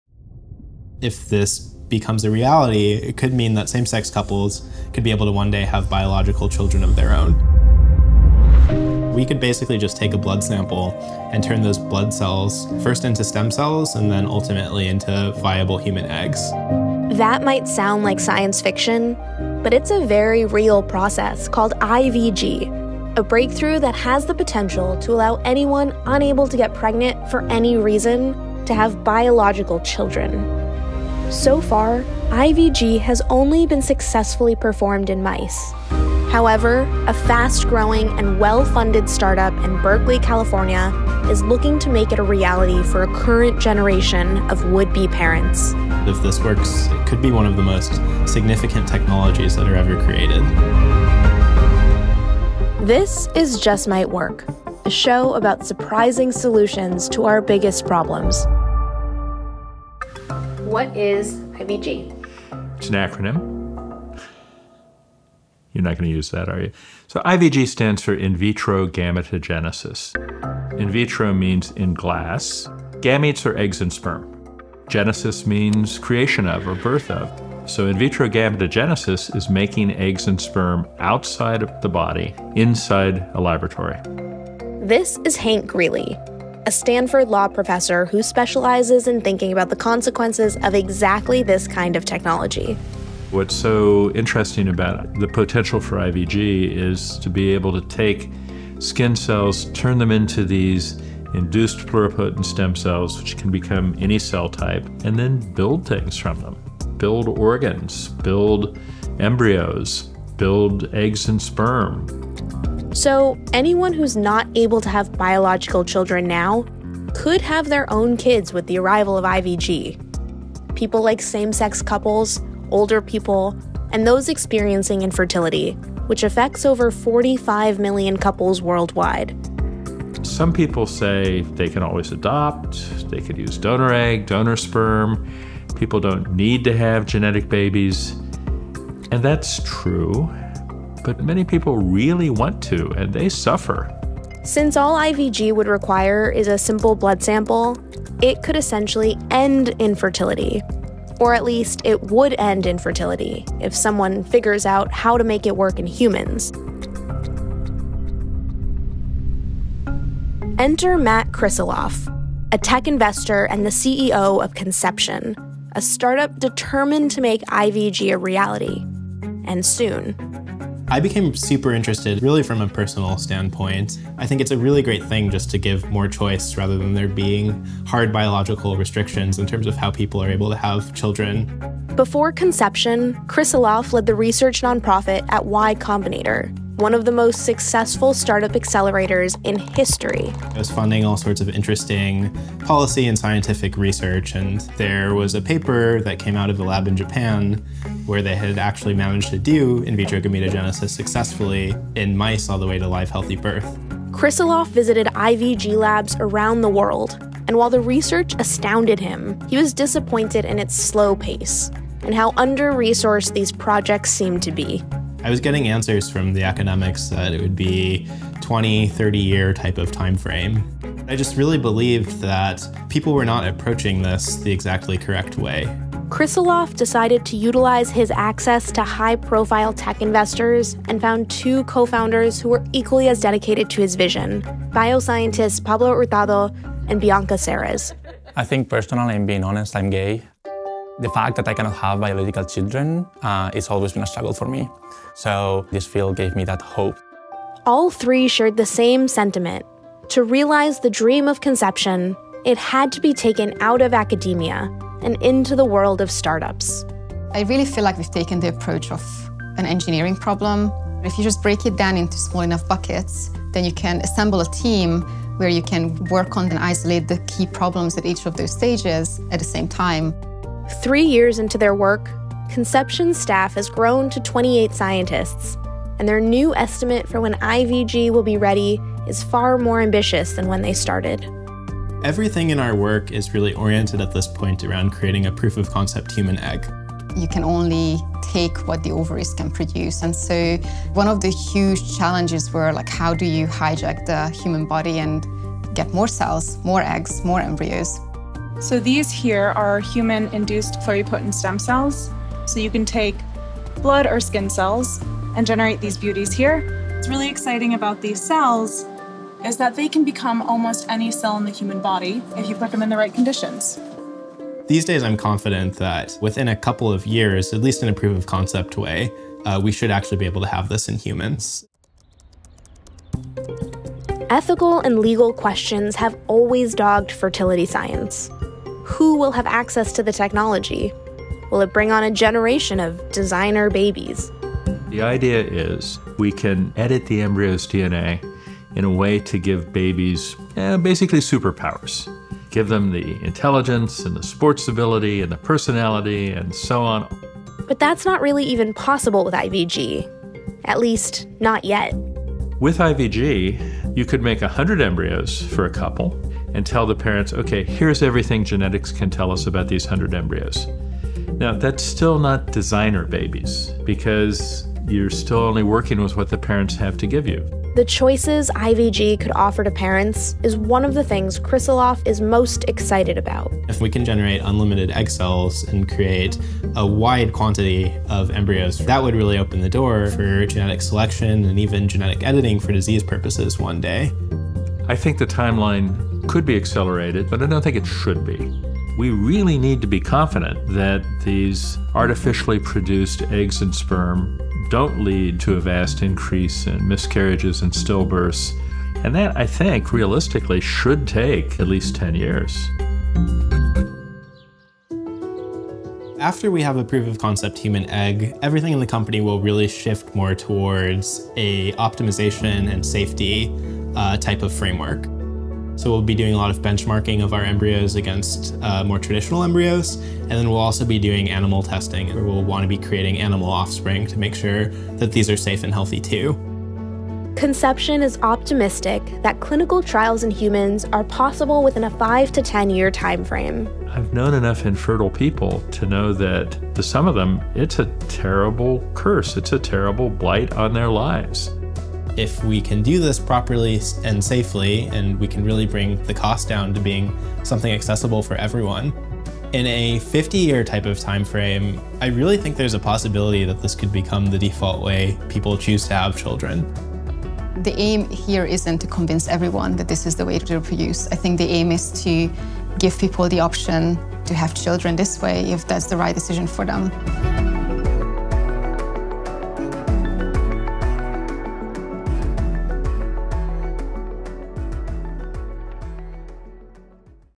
IVG: 10 years until the end of infertility | Just Might Work
“In a 50 year timeframe this could become the default way that people choose to have children.”

In vitro gametogenesis (IVG): It might sound like science fiction, but someday, we may be able to convert human skin cells into viable human egg and sperm cells.

This technology would allow for same-sex couples and those struggling with infertility to have biological children of their own.

But here’s the catch: So far, IVG only has been successfully performed in mice. However, a fast-moving startup in Berkeley, California is on the road to making it a reality.

Read more of our stories on infertility:
Can a lab-grown uterus treat infertility?